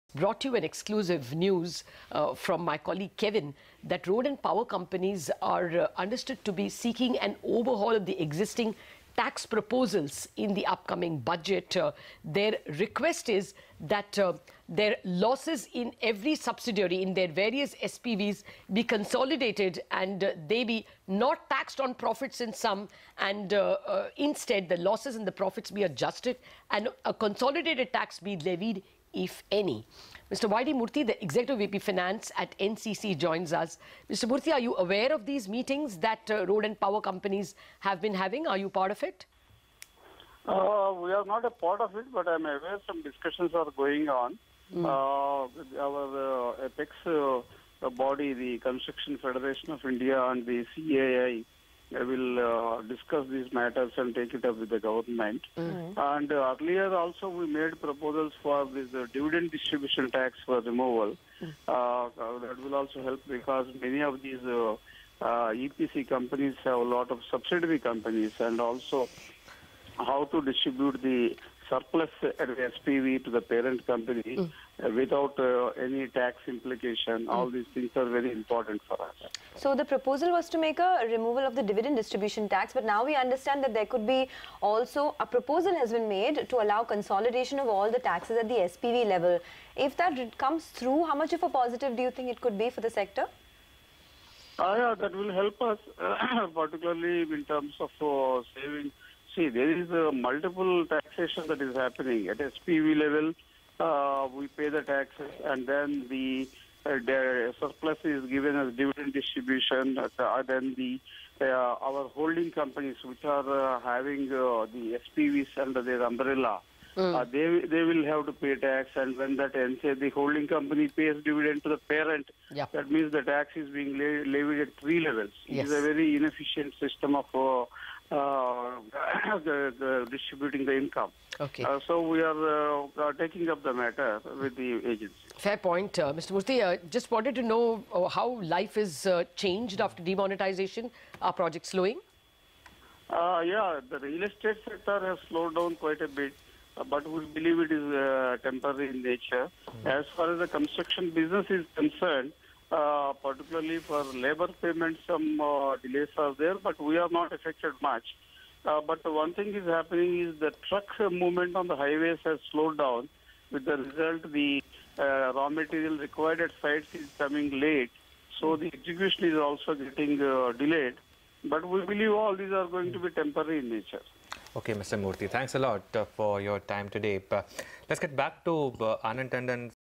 Profit Is Currently Taxed At Multiple Levels: NCC
YD Murthy, Exec VP-Fin, NCC states that profit is currently taxed at multiple levels.